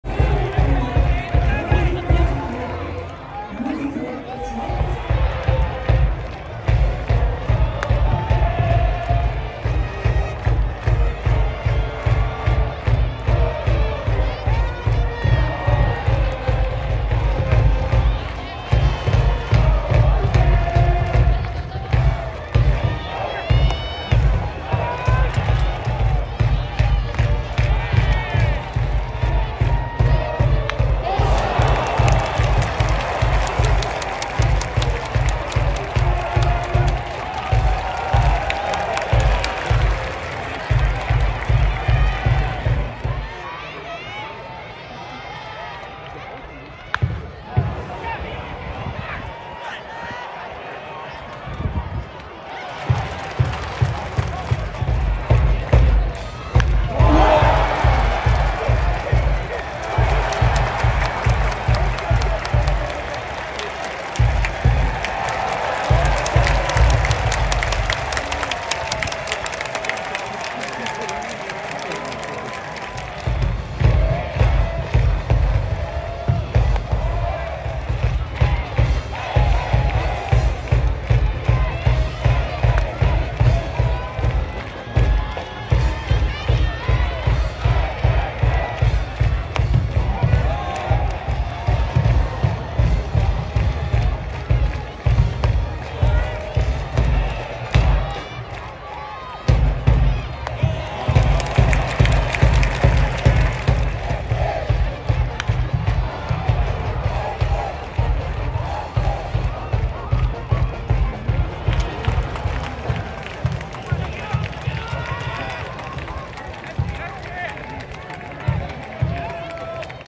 2023年ドラフト1位候補 明治大学 上田希由翔 大学4年春 リーグ戦での打席(対慶應義塾戦)
2023年4月23日　東京六大学野球春季リーグ戦　明治大学対慶應義塾大学　2回戦